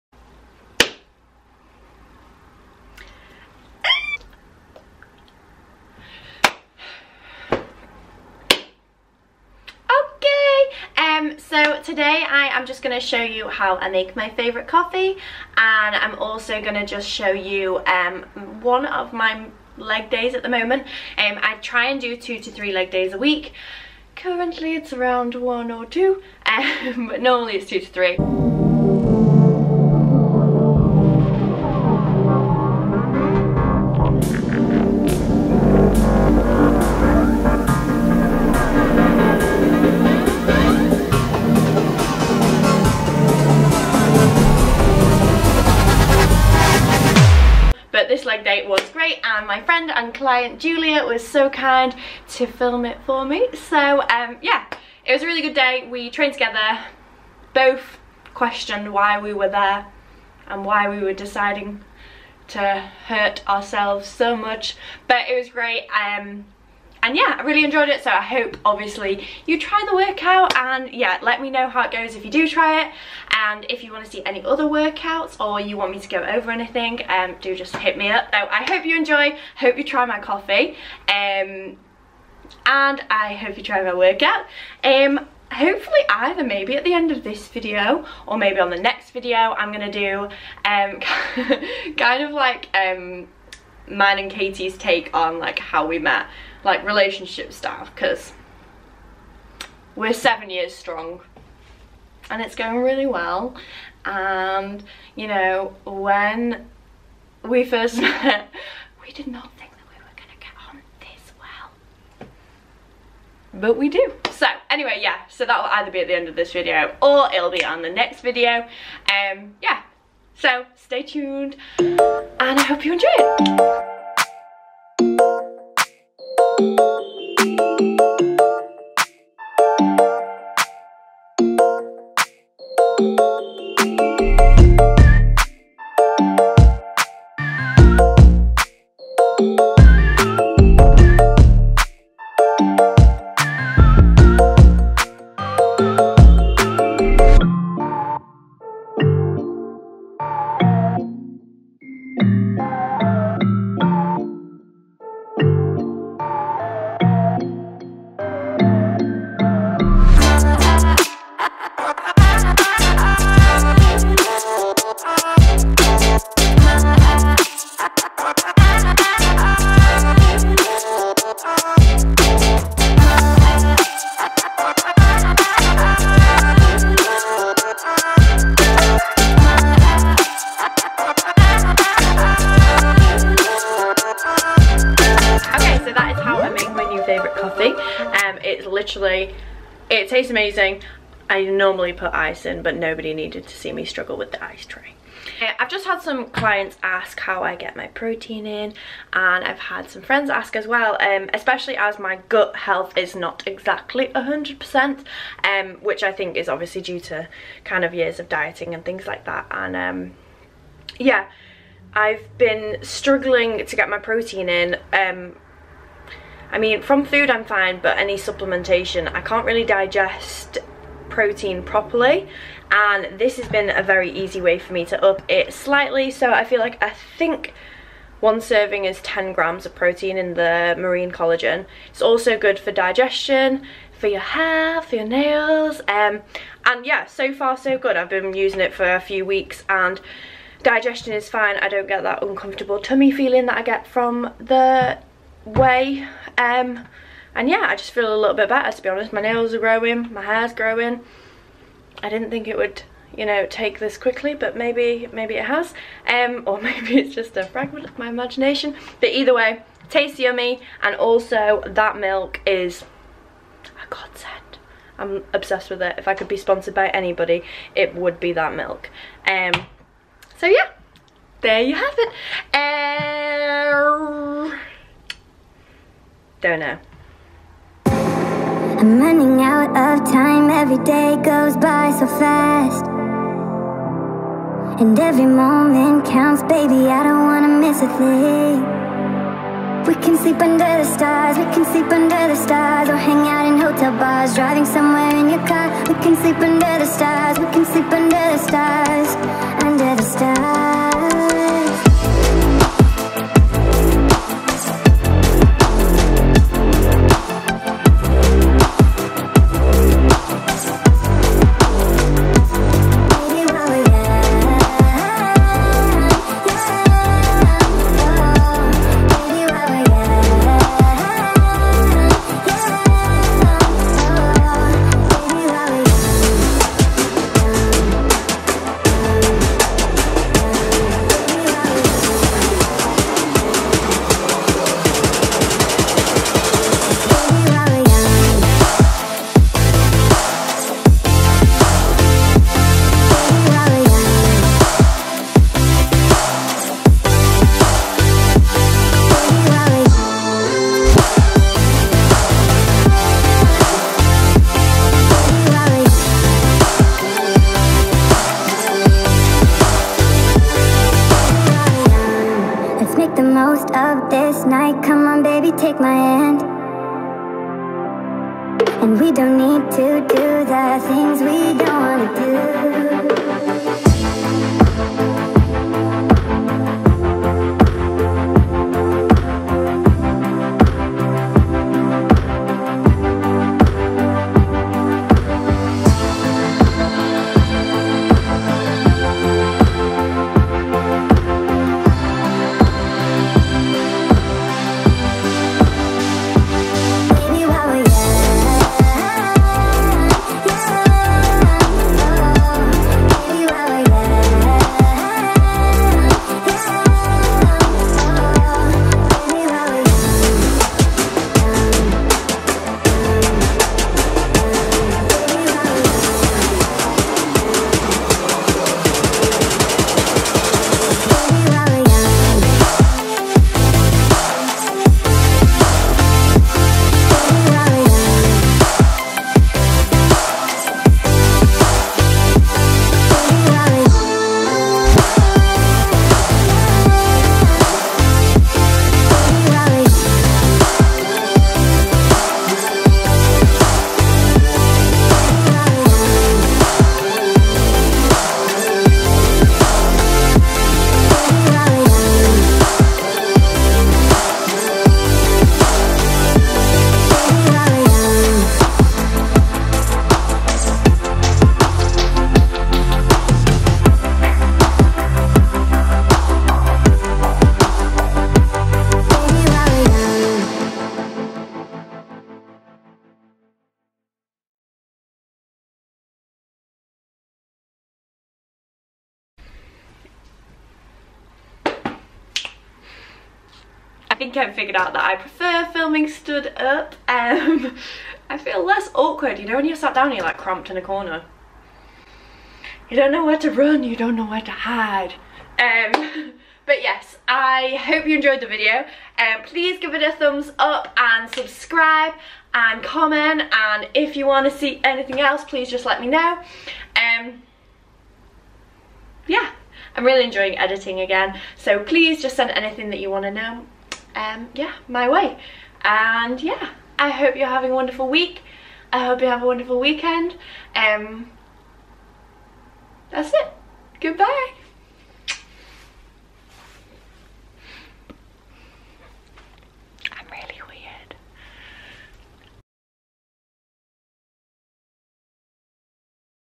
Leg workout | How to make my favourite iced coffee
Hey guys!! I feel like I'm getting a little more confident woohoo, this is my new favourite coffee using marine collagen (I have a cafetiere and usually use lavazza or coffee from independent places in Sheffield) and one of my leg days from this week!

Lower Body workout:
Abductor 2x12 light 2x8 heavy
Hip Thrusts 3x10 moderate
Split Squat 2x8
Smith Machine Squat 2x12
Lying Leg Curl Superset Reverse Lunge 3x12

All music used is from a paid subscription service Epidemic Sounds.